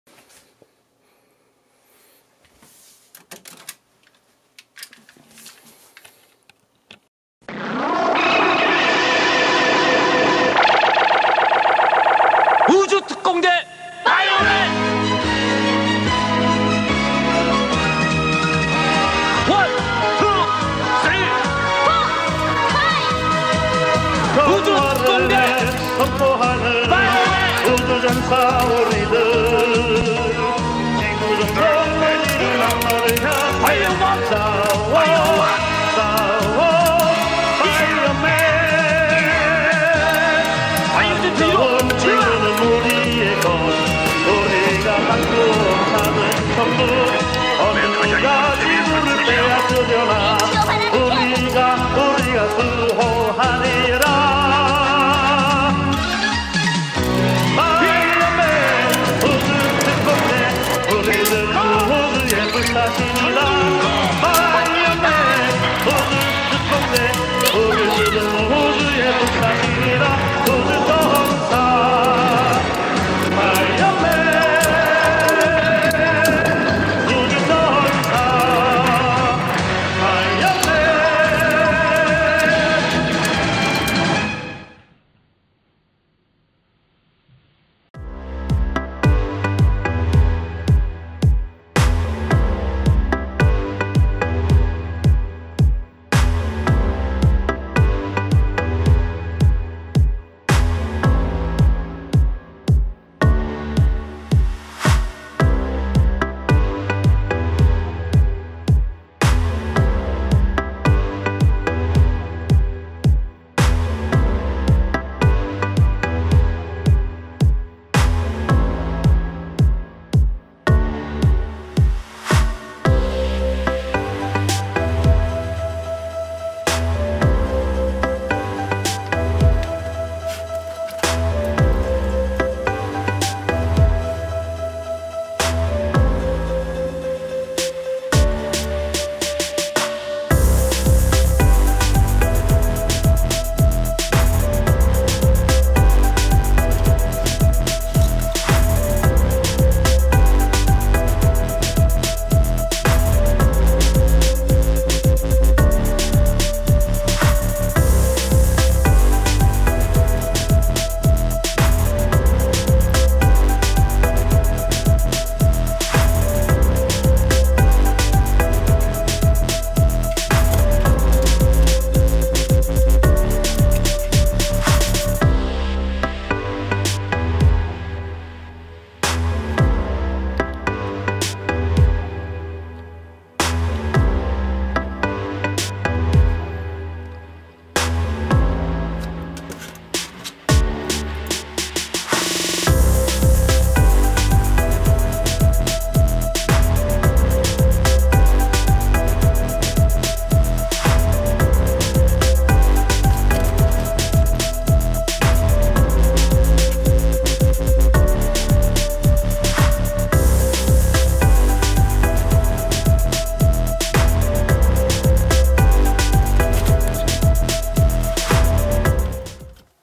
추억의 우주특공대 바이오맨 장난감 먼지 털어주기! / 超電子バイオマン DX초합금 바이오로보
오늘은 오래된 바이오맨 장난감을 가지고 왔어요!
다음은 어떤 것을 가지고 올까요?

(1) DX초합금 바이오로보
- DX Chogokin Bio Robo / DX 超合金 バイオロボ

(2) 초합금 초전대 바이오맨 피규어 / 우주특공대 바이오맨 피규어
- Chogokin Super Electronic Bioman / 超合金 超電子バイオマン